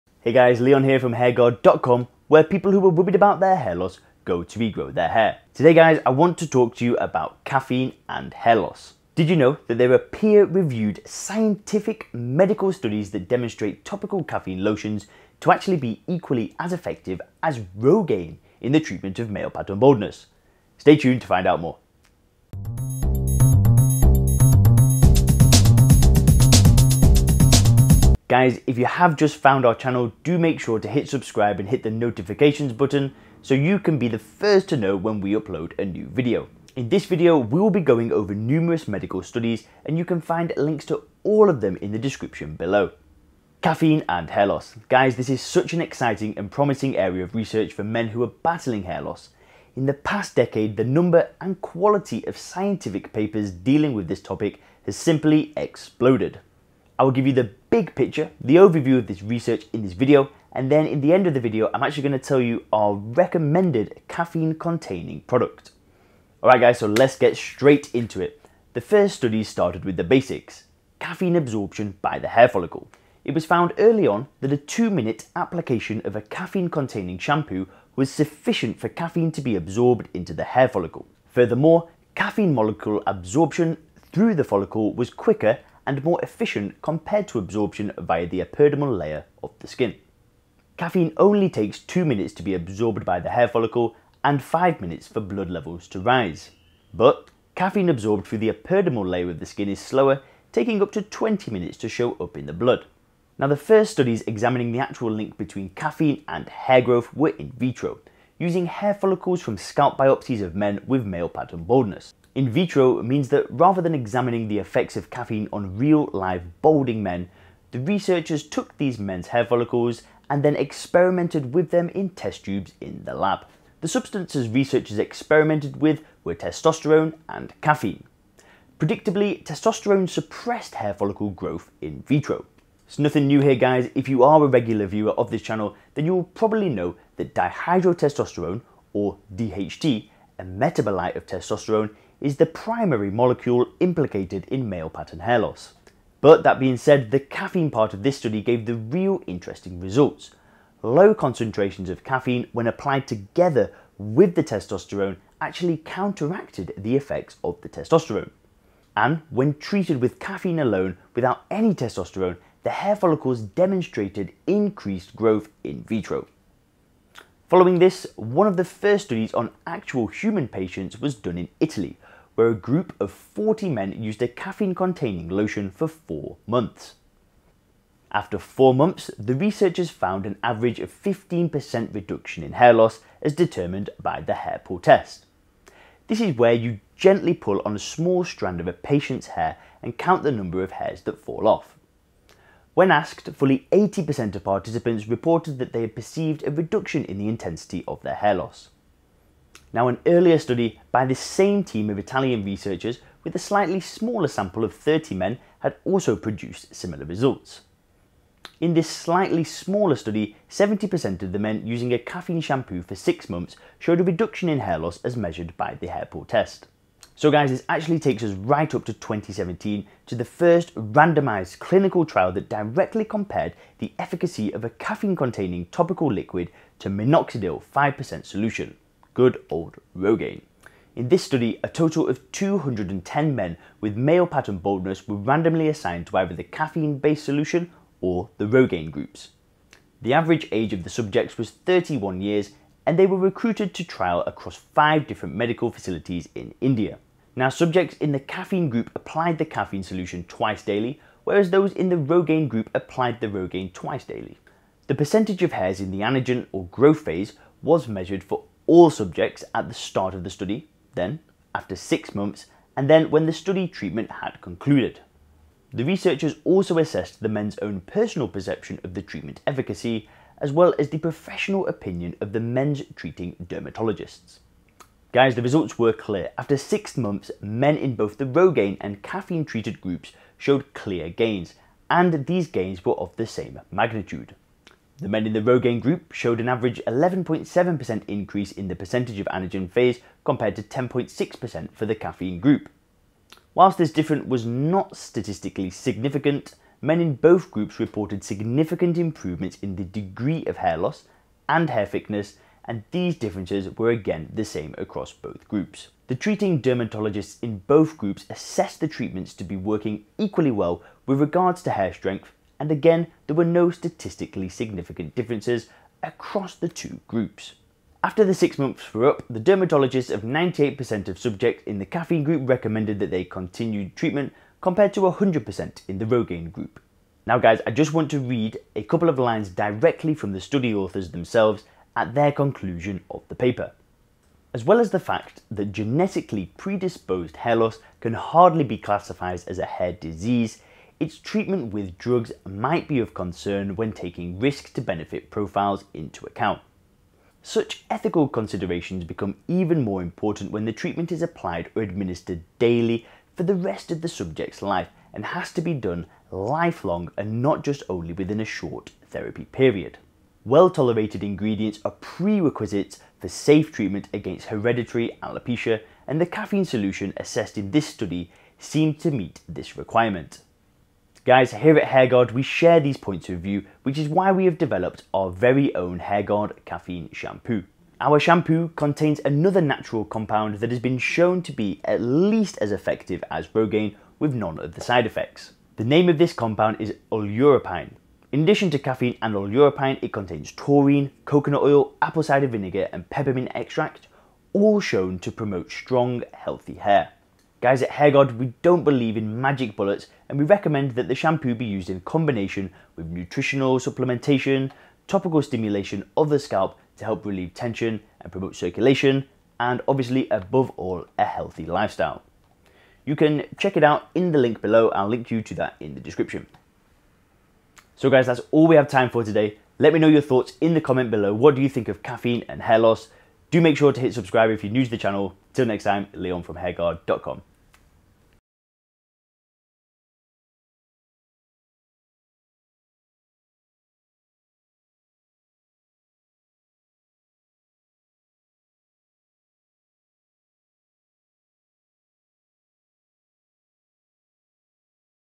Caffeine & Hair Loss - As EFFECTIVE as ROGAINE?
In this video we discuss caffeine and hair loss - is it as effective as Rogaine? Find out how caffeine works on hair, learn about the studies that have shown it's effectiveness, and get the answers to your questions about caffeine and hair loss when you watch the full video!

Transcription

Caffeine and hair loss. Guys this is such an exciting and promising area of research for men who are battling hair loss. In the past decade the number and quality of scientific papers dealing with this topic has simply exploded. I will give you the big picture, the overview of this research in this video, and then in the end I’m going to tell you about our recommended caffeine-containing product.

Alright. So let’s get straight into it. The first studies started with the basics: caffeine absorption by the hair follicle. It was found early on that a two minute application of a caffeine-containing shampoo was sufficient for caffeine to be absorbed into the hair follicle.

Furthermore caffeine molecule absorption through the follicle was quicker and more efficient compared to absorption via the epidermal layer of the skin. Caffeine only takes two minutes to be absorbed by the hair follicle, and five minutes for blood levels to rise. But caffeine absorbed through the epidermal layer of the skin is slower, taking up to 20 minutes to show up in the blood.

The first studies examining the actual link between caffeine and hair growth were in vitro, using hair follicles from scalp biopsies of men with male pattern baldness. In vitro means that rather than examining the effects of caffeine on real live balding men, the researchers took these men’s hair follicles and experimented with them in test tubes in the lab.

The substances researchers experimented with were testosterone and caffeine. Predictably,
testosterone suppressed hair follicle growth in vitro. Nothing new here – if you are a regular viewer of this channel you will probably know that Dihydrotestosterone or DHT, a metabolite of testosterone, is the primary molecule implicated in male pattern hair loss.